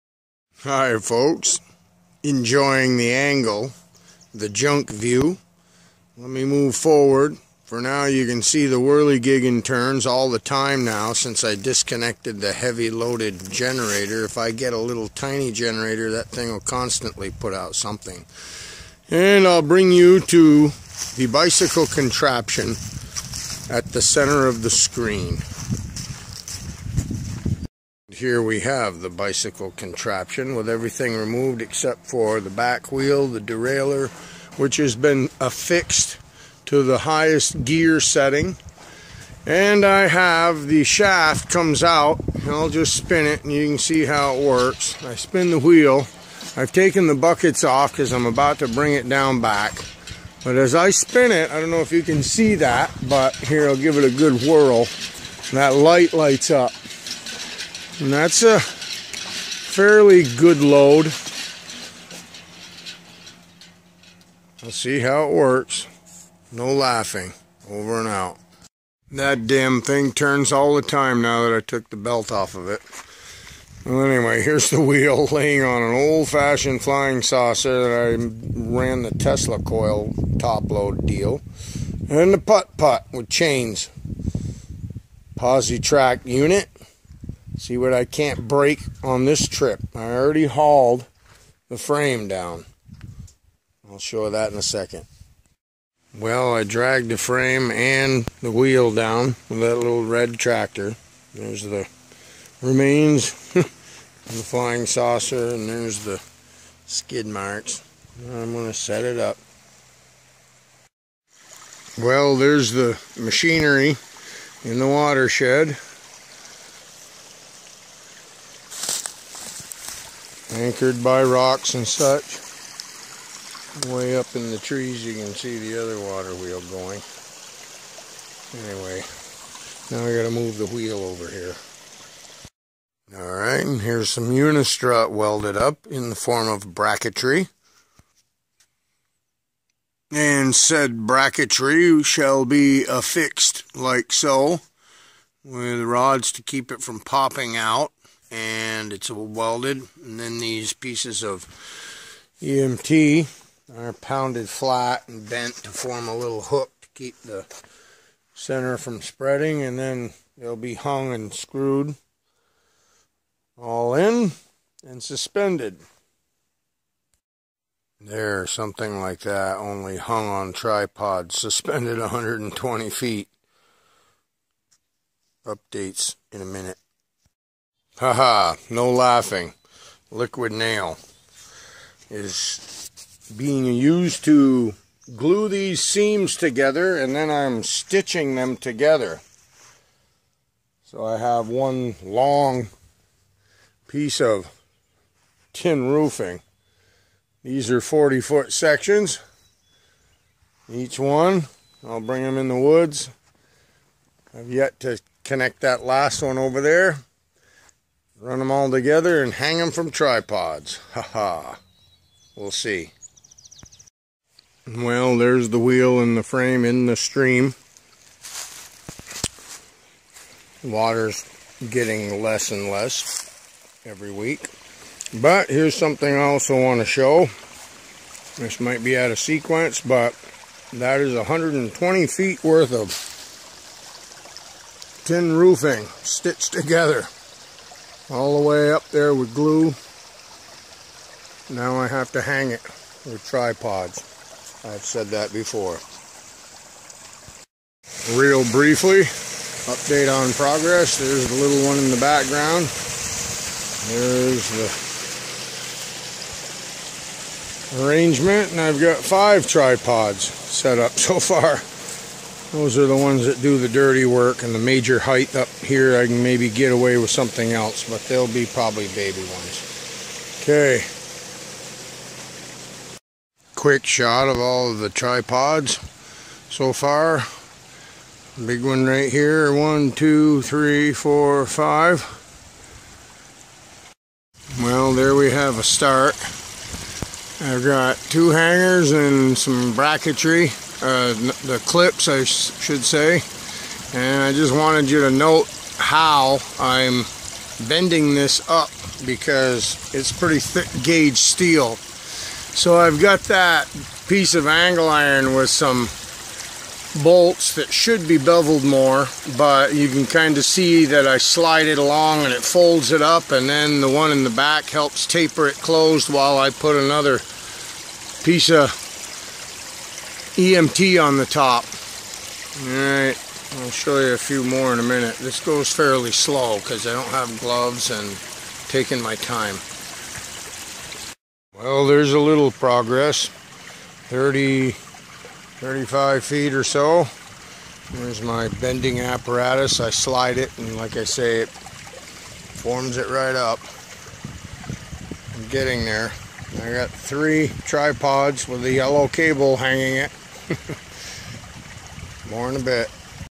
waterwheel part II of III (Bicycle drive mechanism and sleuce build)
part two showing the sleuce trough suspension and such, Wheel in the water shed now, plus some tidbits down in the woods.